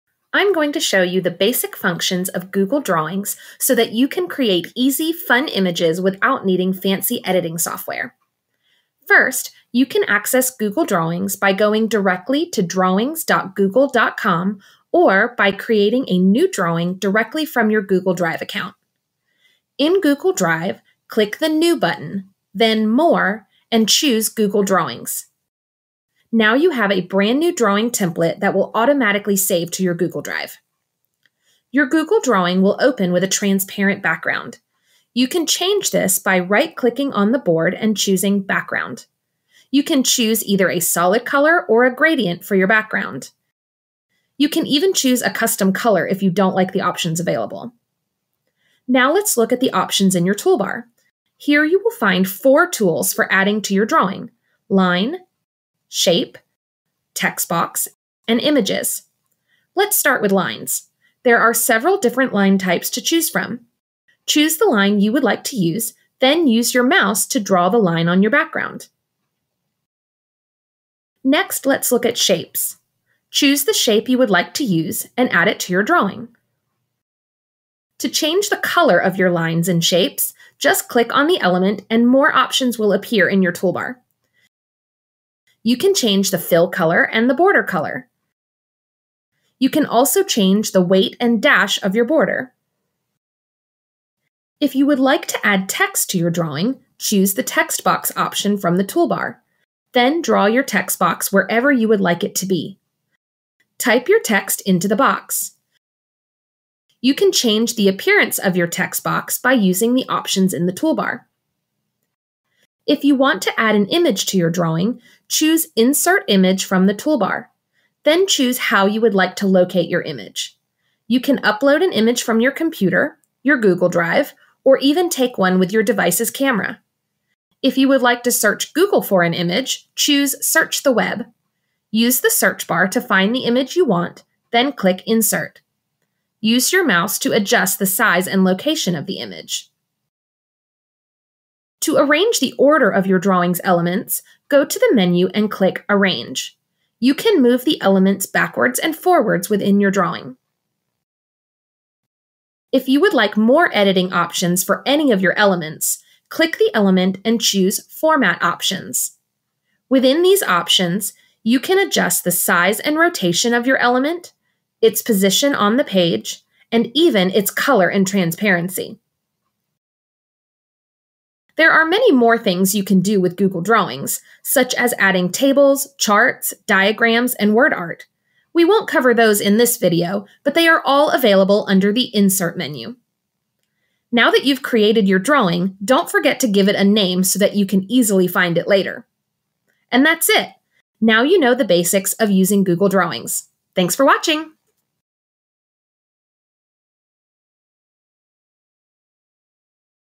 Google Drawings Basics
Learn the basics of Google Drawings with this quick and easy tutorial!
- Change your background
- Add lines, shapes, text boxes, and images
- Change the colors and borders of elements
- Adjust size, rotation, and position of elements

Created with Screencastify and Wondershare Filmora X.